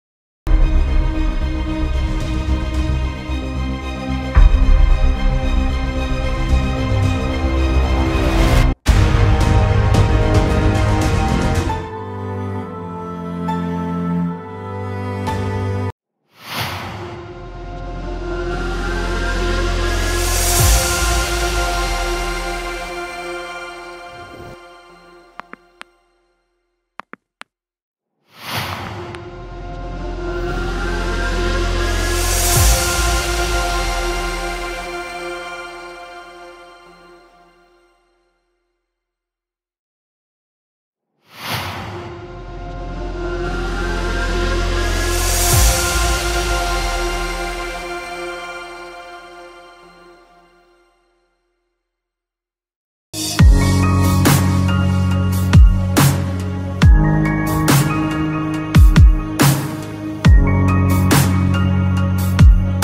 Free After Effects Intro Template : Clean 3D Logo Reveal Intro Template for After Effects
▬▬▬▬▬▬▬▬▬▬  ∂σωηℓσα∂ ℓιηк  ▬▬▬▬▬▬▬▬▬▬▬▬▬

▬▬▬▬▬▬▬▬▬▬  Instructions  ▬▬▬▬▬▬▬▬▬▬▬▬▬
1) Extract the template File using Winrar

2) Replace my logo with your logo

3) Edit the text to your liking

4) Render and Enjoy!